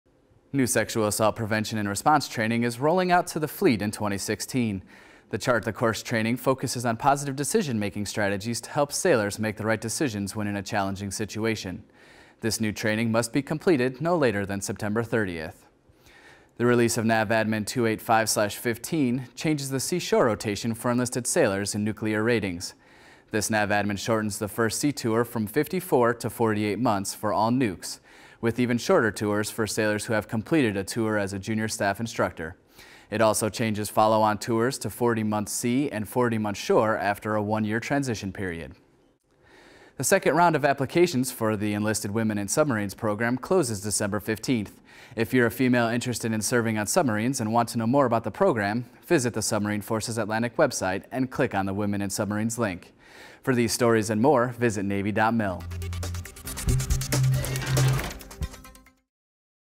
Headlines for Friday, December 11, 2015 (HL 11)
New SAPR training announced, Sailors in nuclear rates get changes to sea, shore rotation, The deadlines for female applications for women on submarines approaches.